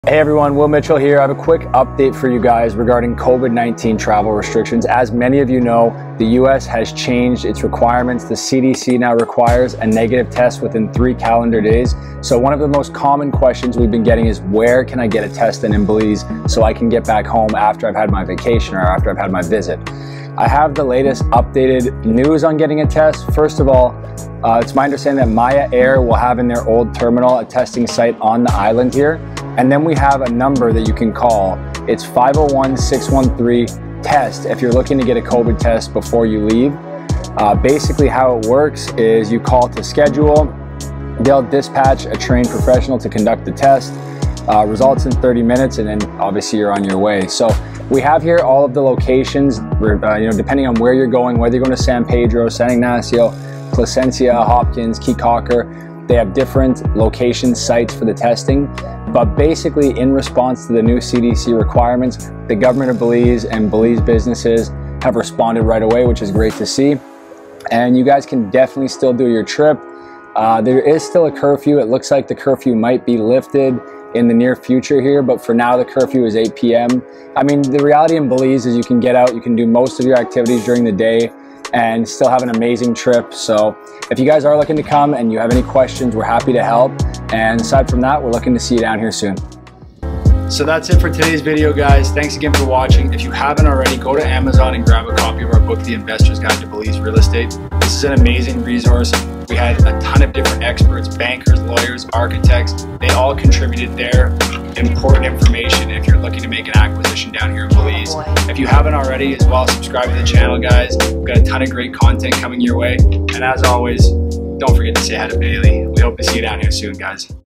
BELIZE Covid Travel Update - January 2021
If you're looking to travel to Belize and are wondering how you can do so with the latest restrictions from the CDC look no further! Will Mitchell & the RE/MAX team break down where & how you can get tests done in Belize. If you're travelling from the US to Belize this is your latest update!